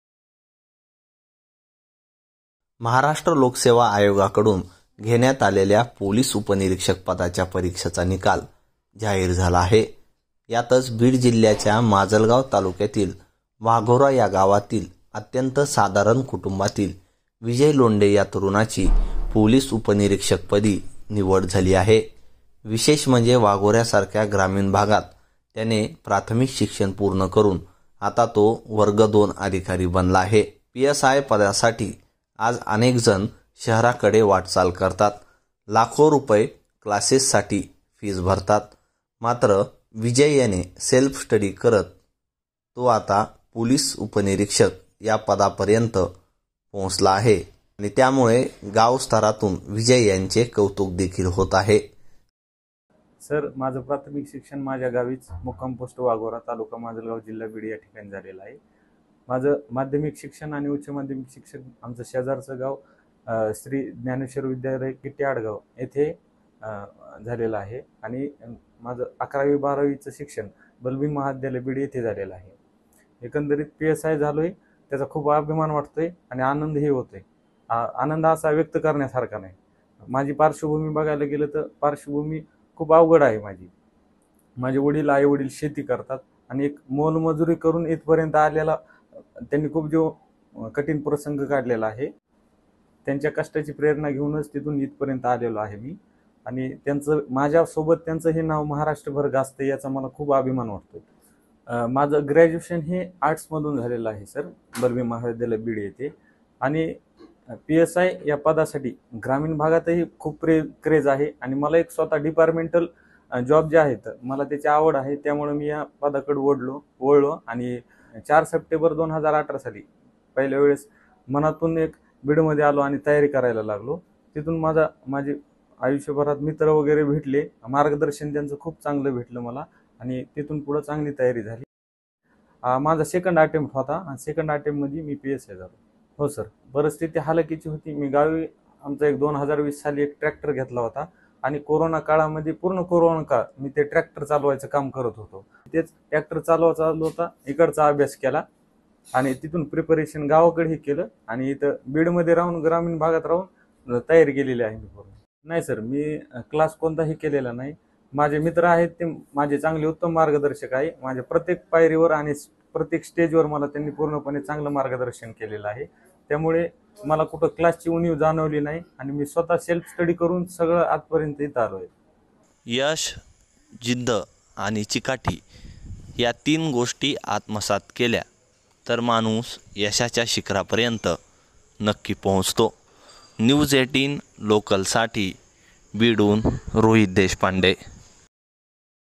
Success Story। शेतात काम करत केला अभ्यास, शेतकरी पुत्र झाला फौजदार, Video
महाराष्ट्र लोकसेवा आयोगाकडून घेण्यात आलेल्या पोलीस उपनिरीक्षक पदाच्या परीक्षेचा निकाल नुकताच जाहीर झाला. यामध्ये अनेक सामान्य कुटुंबातील तरुणांनी यश मिळवलं आहे. बीड जिल्ह्यातील वाघोरा येथील शेतकरी पुत्र विजय कोंडिबा लोंढे दुसऱ्याच प्रयत्नात फौजदार झाला आहे. वडिलांसोबत शेती करतच विजयनं पोलीस उपनिरीक्षक परीक्षेचा अभ्यास केला. जिद्द आणि मेहनतीच्या बळावर विजयनं मोठं यश संपादित केलं असून त्याचं सर्वत्र कौतुक होत आहे.